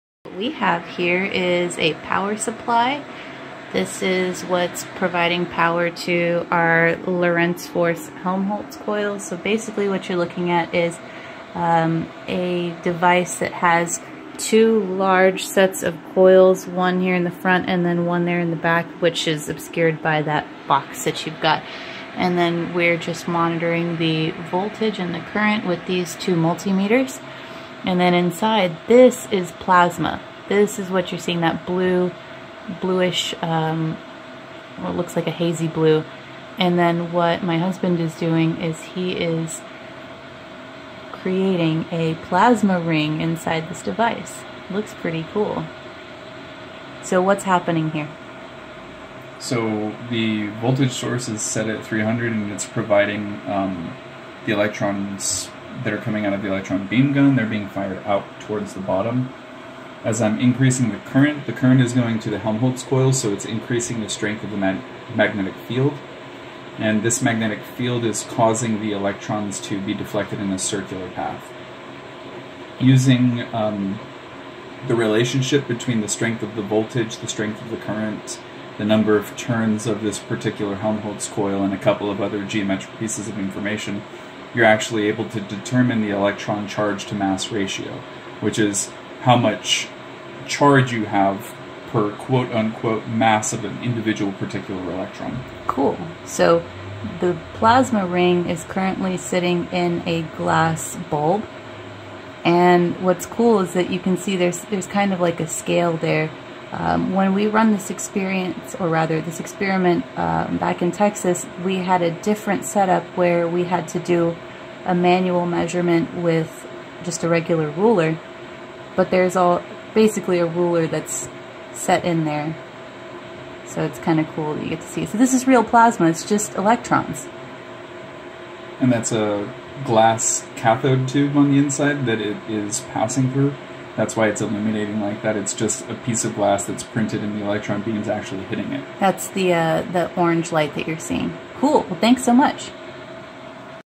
Helmholtz coils deflecting electron plasma beam
This experiment demonstrates the Lorentz force of a magnetic field on electrons in a plasma beam. The magnetic field causes electrons to be deflected in a circular path based on the current passing through the copper coils.